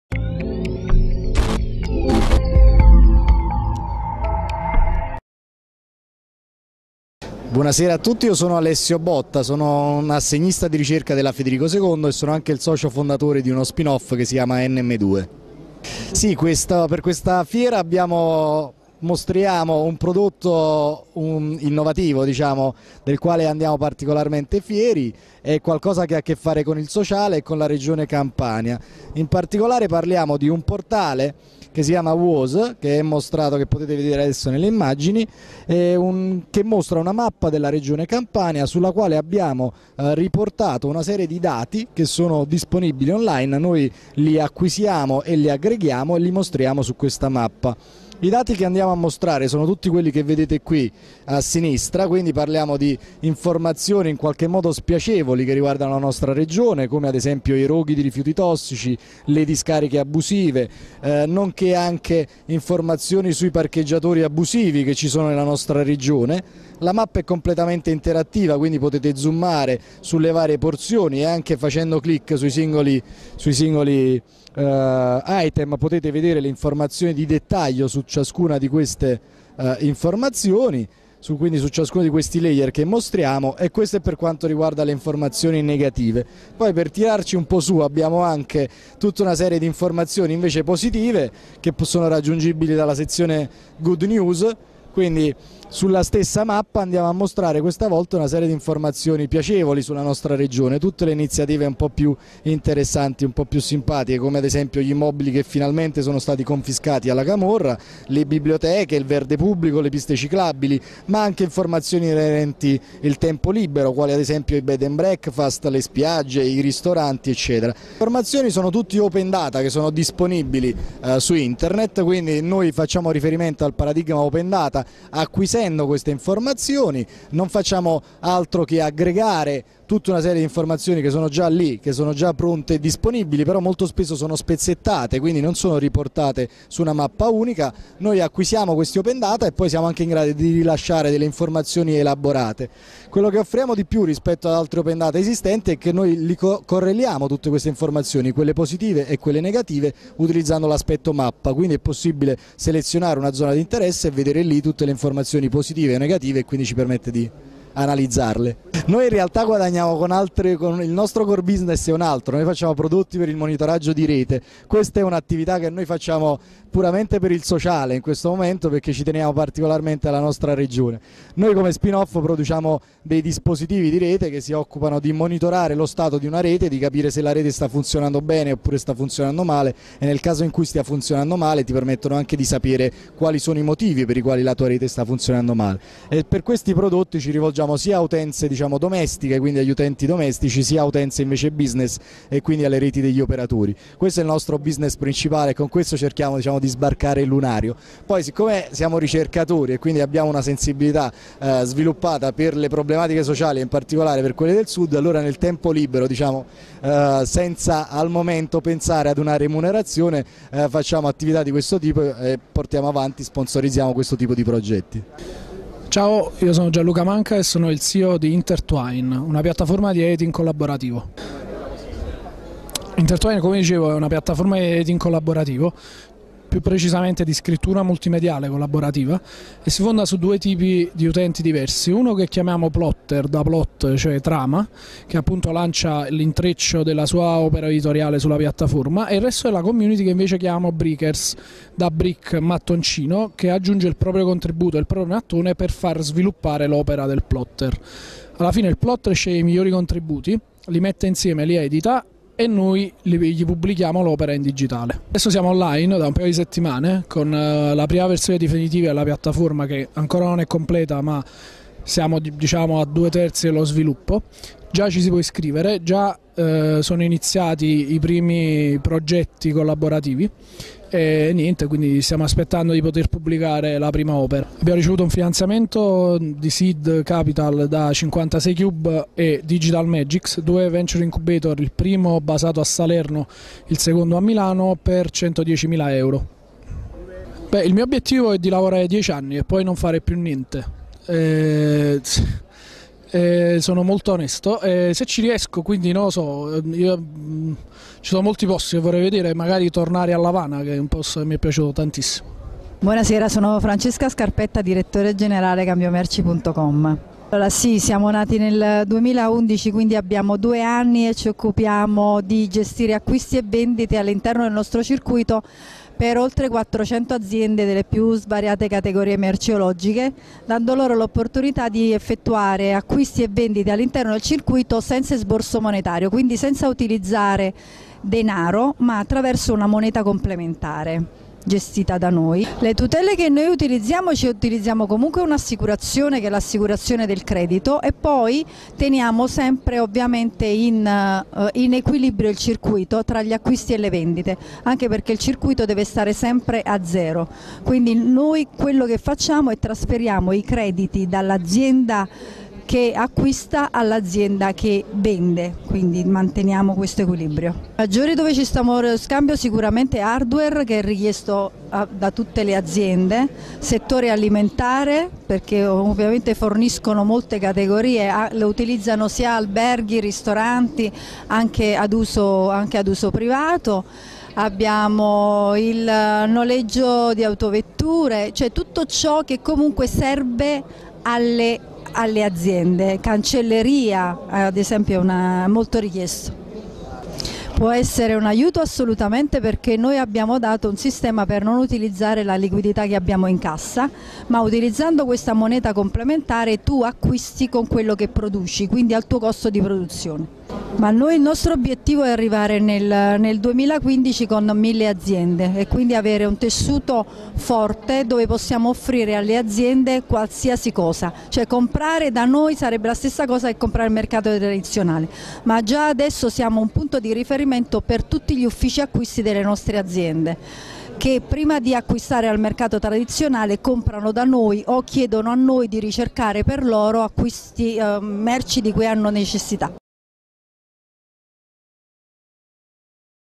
Videointervista. Smau Napoli mette in vetrina le startup della Campania
Il 12 e 13 dicembre  a Napoli i protagonisti sono stati  l'innovazione ed il networking, elementi ricorrenti e che contraddistinguono l'ecosistema delle startup
Abbiamo incontrato tra le tante startup presenti tre esperienze molto interessanti: WAS (World Around uS), sviluppato da NM2, raccoglie e mostra informazioni sulla Regione Campania. WAS ti mostra la faccia brutta (badnews), e quella bella (goodnews). Intertwine è una piattaforma per creare e pubblicare opere multimediali condivise. CambioMerci network di imprese e professionisti che scambiano all'interno del sistema B2B prodotti e servizi, è una piattaforma di baratto 2.0 dove le aziende, a dispetto della crisi, uniscono forze, competenze e know how per scambiarsi beni e servizi.